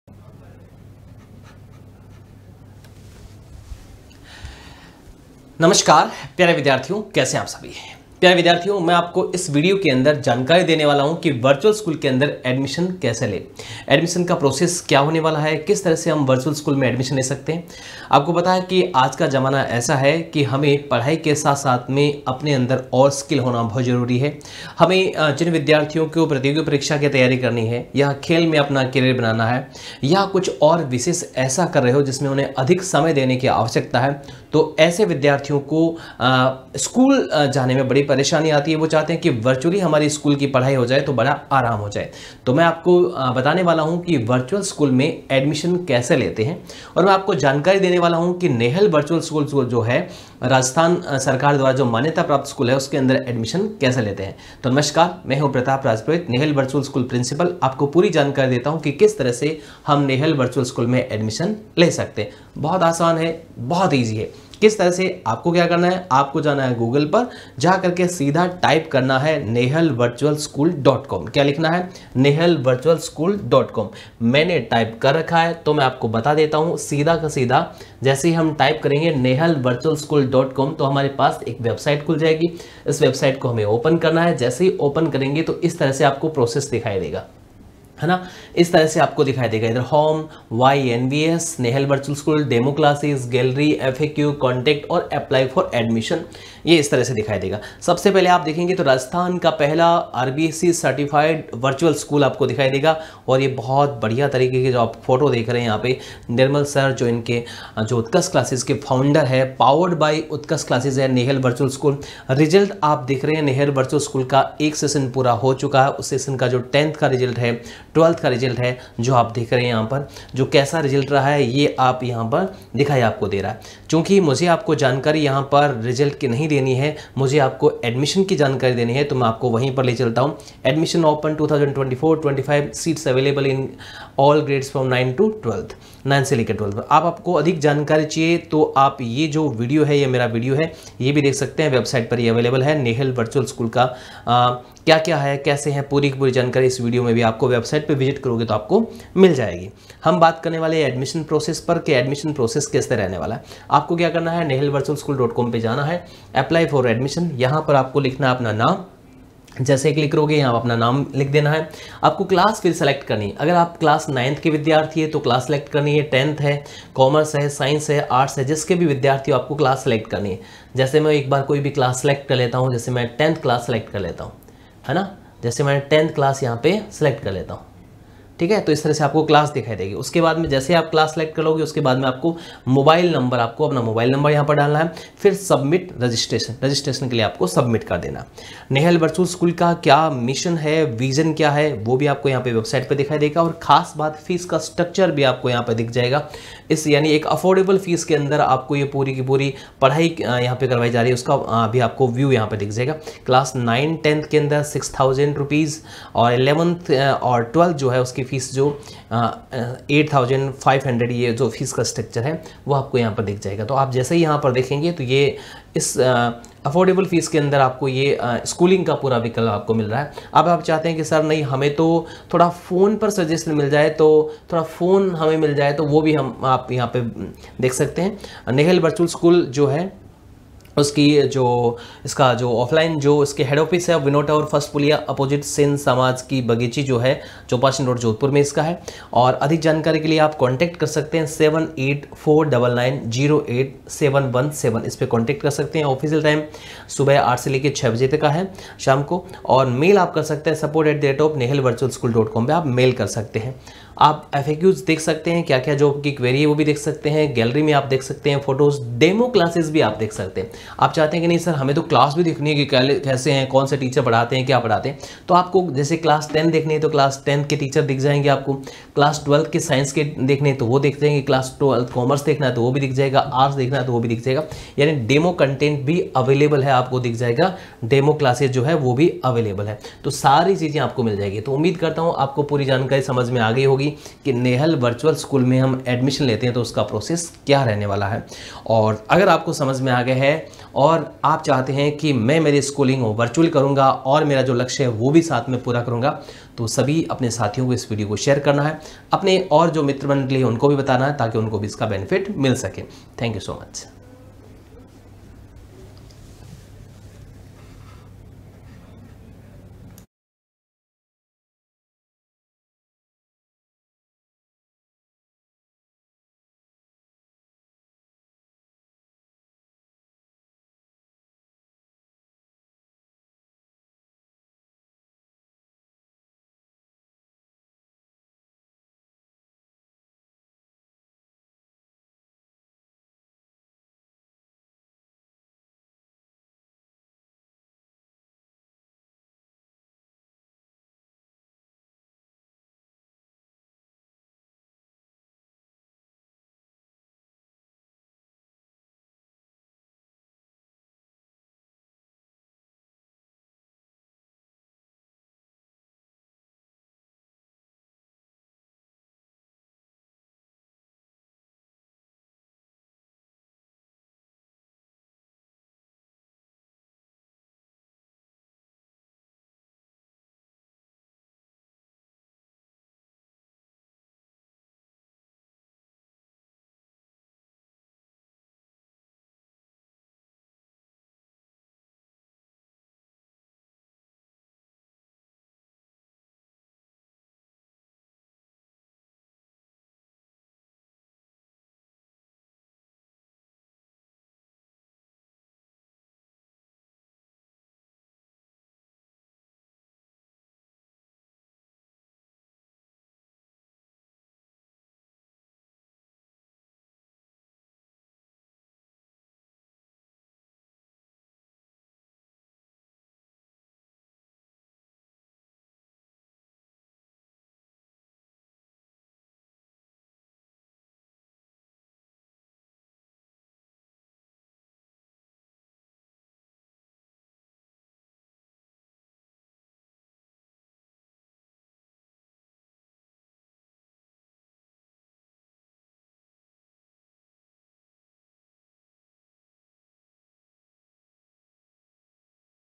Nehal Virtual School Admission Process | Complete Details | Pratap Sir
In this video, Pratap Sir provides a comprehensive guide on the admission process for Nehal Virtual School.